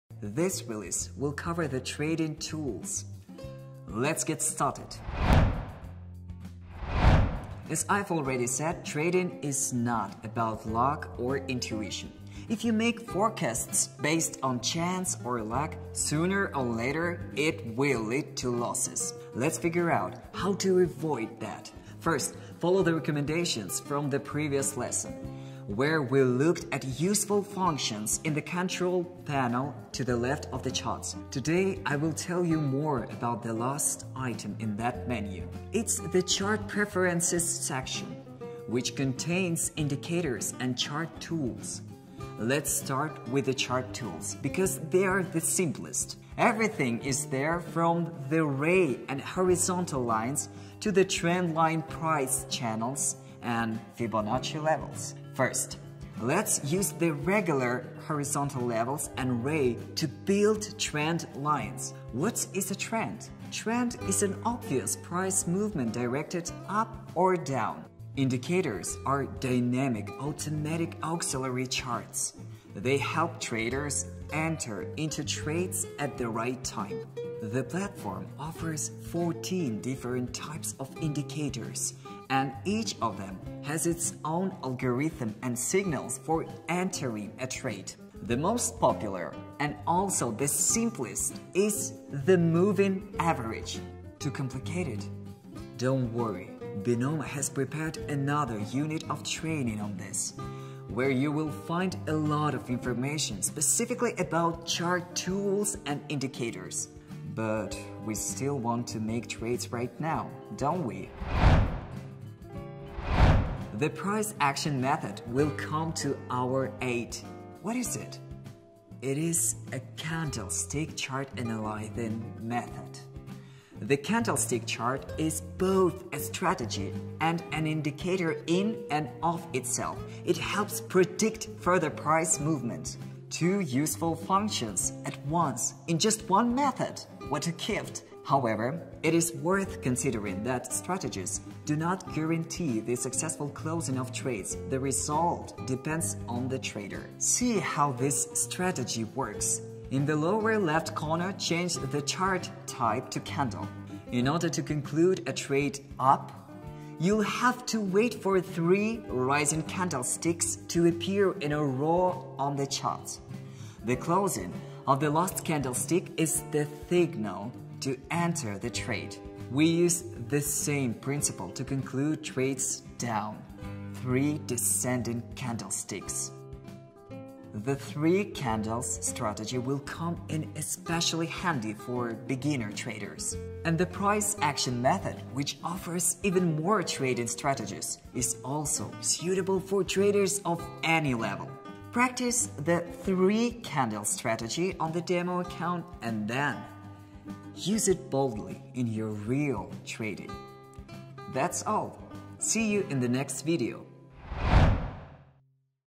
Binomo - Trading chart [Step 5] | Crypto Binary Options
In addition to the previous video, in the following one we want to share the trading tools that will help you to close the trade successfully. You will learn how to make the most of using the chart preferences elements - indicators and chart tools.

Watch our demonstration examples of using these features and don’t miss a thing!